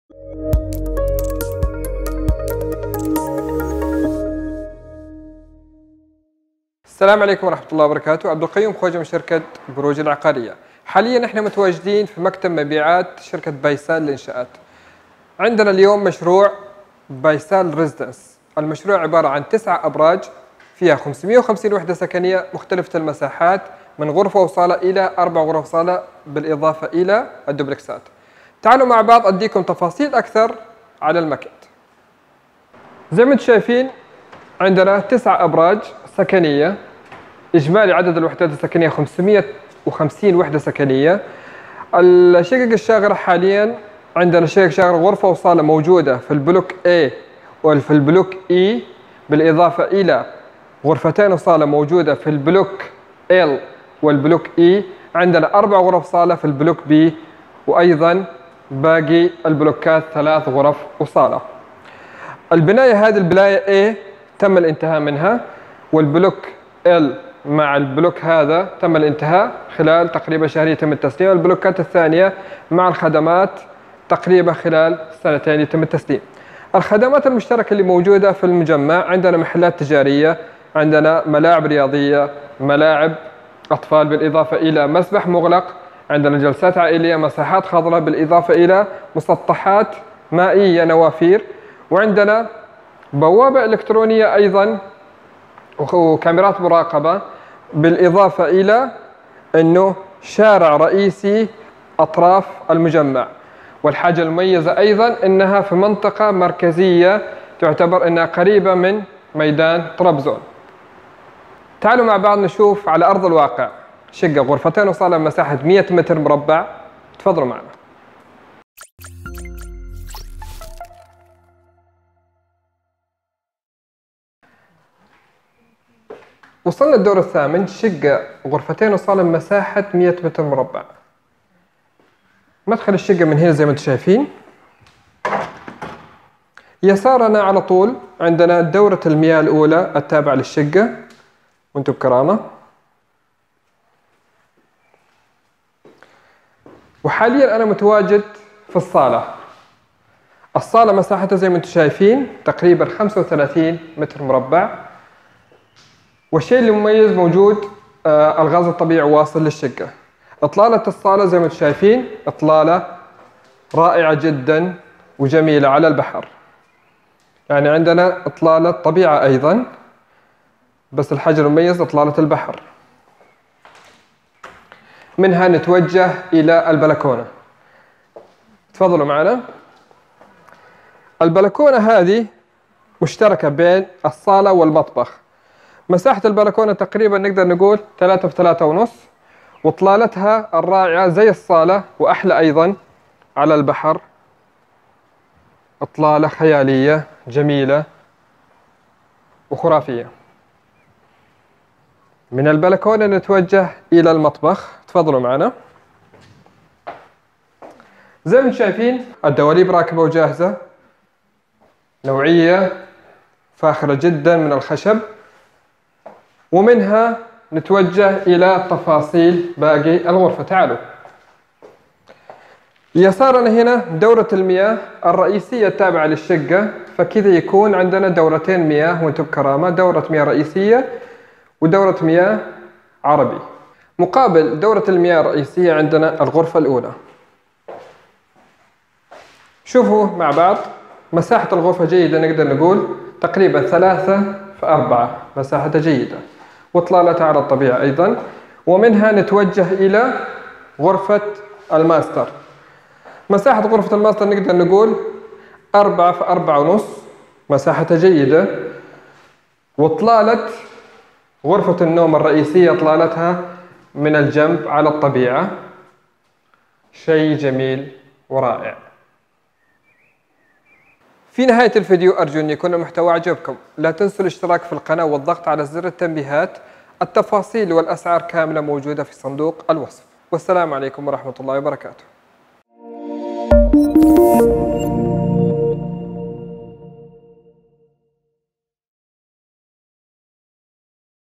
تملك في اكبر المشاريع السكنية وبدفعات سهلة في طرابزون في منطقة مرتفعة وحيوية
يطل عليكم مشروع (بايسال 1461 ريزيدنس) الذي يقع في
منطقة بوزتبه -جوكورجاير  الوجه الحديث لعروس الشمال التركي
مدينة طرابزون.
المشروع في منطقة حيوية وسط البلد
يبعد عن المطار 8 دقائق
وعن الميدان 5 دقائق
المشروع عبارة عن 10 ابراج سكنية
بها شقق  مختلفة المساحات  : --

( 1+1 /   85     متر مربع
 2+1   /  109 / 117 / 135  متر مربع
 3+1   / 162  /  169  متر مربع
 4+1   /195  متر مربع
 دوبلكسات /  385   /   460  /  680     متر مربع

 مساحة الارض للمشروع ( 29.000  متر مربع
ـ يتكون المشروع من ( 10 ابراج سكنية مطلة البحر وعلى الطبيعة )
-550 وحدة سكنية
ما يميز المشروع بالدرجة الاولى اطلالتها السحرية على البحر والطبيعة .
وقربها من وسط البلد ( DOWN TOWN )
موجود بالمجمع :
- مواقف سيارات مغلقة ومكشوفة
- جلسات خارجية .
- العاب اطفال .
- مساحات خضراء .
- ممشى خاص بالمجمع .
- بوابه للمجمع وحراسه امنية وكاميرات مراقبة .
- مصعدين لكل عمارة .
ـ نادي صحي .
-مسبح مغلق
-ساونا
-محلات تجارية
سوبر ماركت
ـ ملاعب رياضية
- فتنس
-ملاعب رياضية خارجية
شلالات ونوافير صناعية
الشقة المعروضة في الفيديو /
 غرفتين وصالة بمساحة /100 متر مربع
الاسعار تبدأ من  :

1+1 / 220.000  ليرة
2+1 / 275.000  ليرة
3+1 / 352.000  ليرة
4+1 /   385.000  ليرة
الدوبلكس / 572.000  ليرة
امكانية الدفع بالاقساط لبعض الشقق التي تكون تحت الانشاء

محمد بخاري
رابط الواتساب المباشر للتحدث معي

عقارات_اسطنبول#
عقارات_تركيا#
الاستثمار_في_تركيا#
شقق للبيع في تركيا
شقق_تركيا#
عقارات_طرابزون#